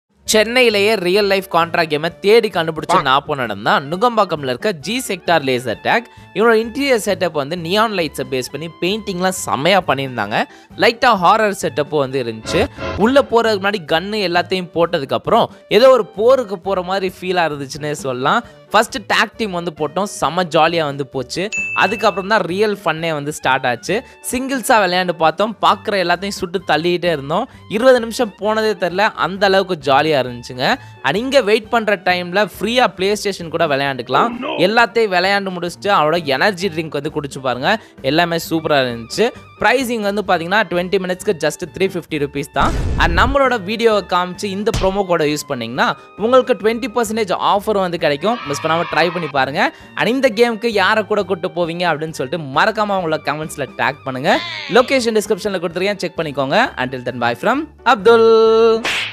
💥🥷 Chennai-ல Laser Shooting Game-ஆ 🔫 Semma Fun Spot 🤯
Location:-
G Sector Lasertag Nungambakkam
Kakani Tower, 15, Khader Nawaz Khan Rd, Thousand Lights West, Thousand Lights, Chennai - 600006.

Price Details:-
20 Minutes - Rs.350
30 Minutes - RS.450
Special 1 Hour Party Combo - Rs.7000

Note:-
Minimum 4 Persons and Maximum 10 Persons Allowed

My Overall Experience:-
- First of all I really loved the interior and the lighting setup.
- Me and my friends enjoyed the game very much, it's not like a normal game.
- You will feel the real shooting experience once you play this game.
- One more additional thing is you can play the PlayStation for free of cost while waiting for the game.
- They have energy drinks also in that Coconut and Sugarcane juices are must try.